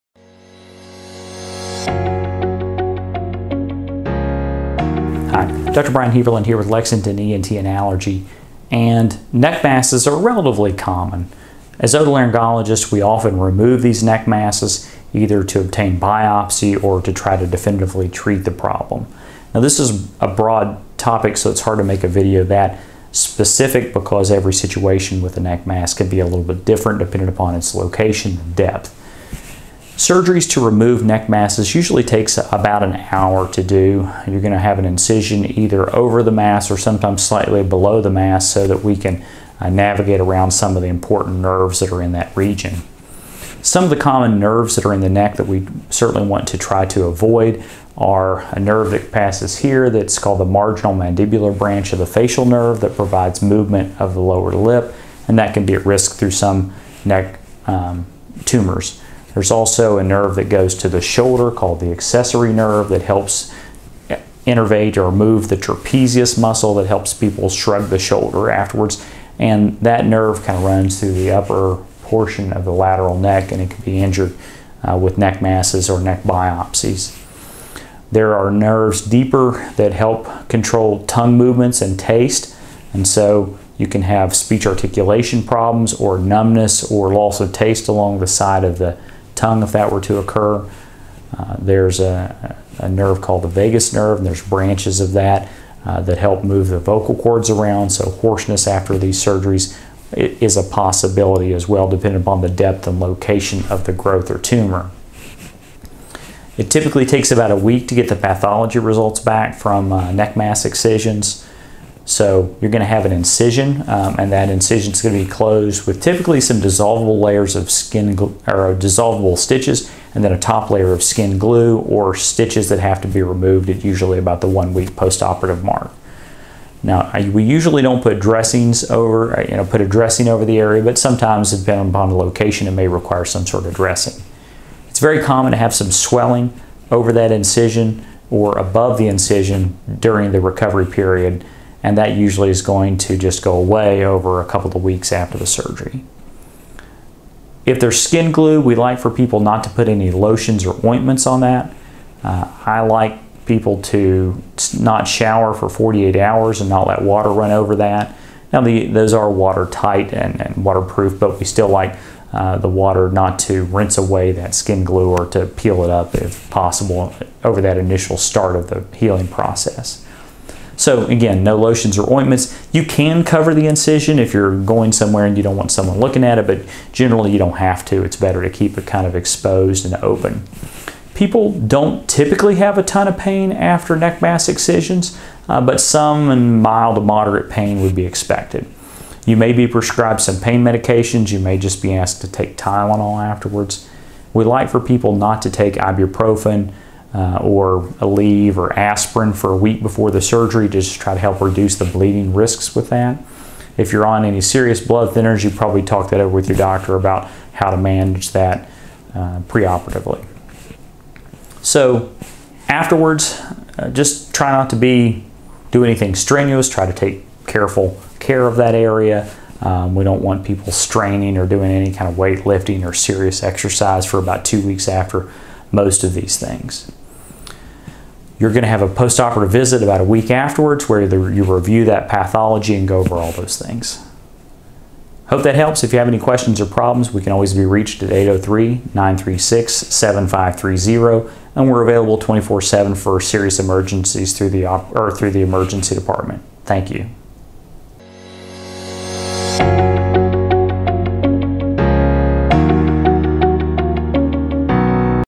Neck Mass Excision Surgery
Dr. Brian Heaberlin, ENT Surgeon in Lexington, SC, provides information regarding neck mass excision surgery.